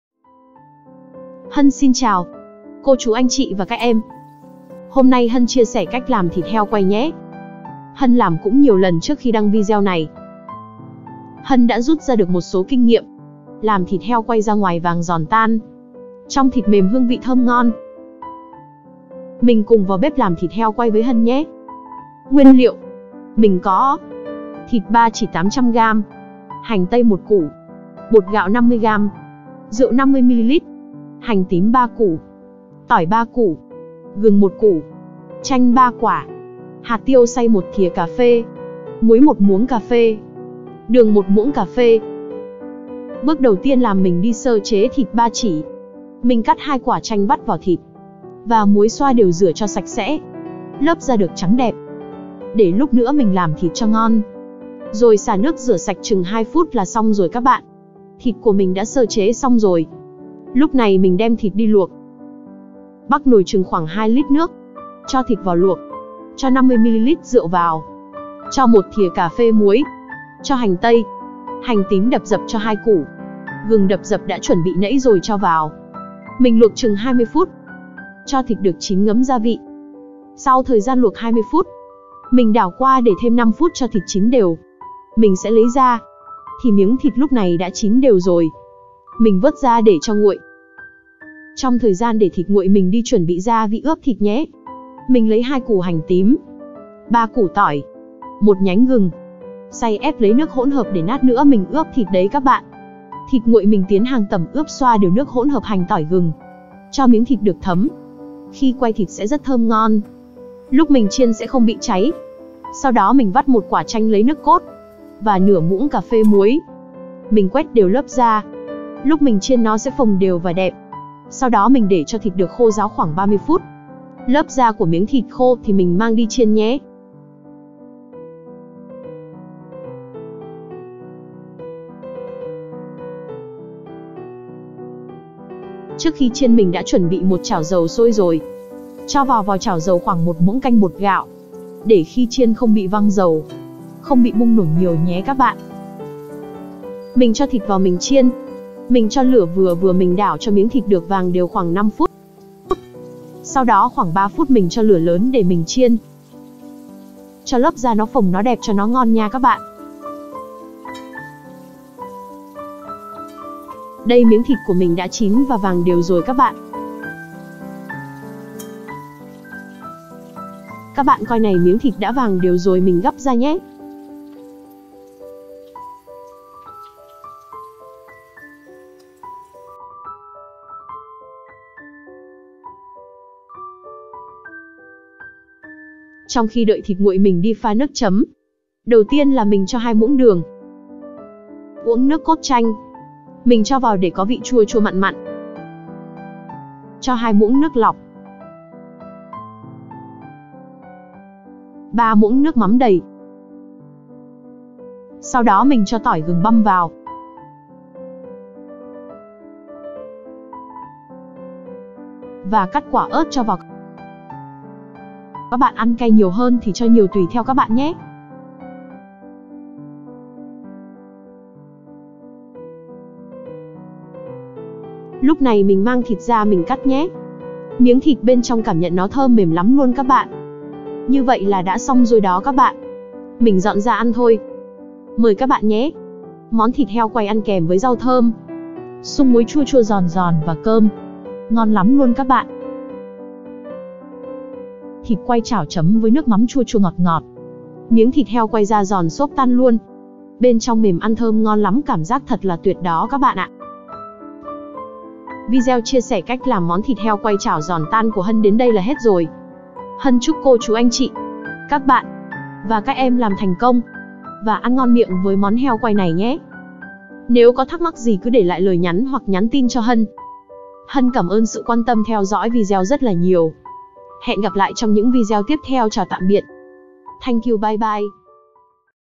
THỊT HEO QUAY CHẢO GIÒN TAN Da Giòn Thịt Mềm Thơm Ngon Đậm Đà |Blog Thỏ fam
Hi chào các bạn hôm nay mình chia sẻ bí quyết làm thịt heo chiên chảo da giòn, thịt mềm thơm ngon thấm gia vị rất đậm đà cho bữa cơm gia đình. Cách làm lại đơn giản tại nhà luôn các bạn. Nếu có dịp các bạn hãy làm theo cách của mình thử nha …..,ngon lắm………Heoquaychảogiònngon Blogthỏfam#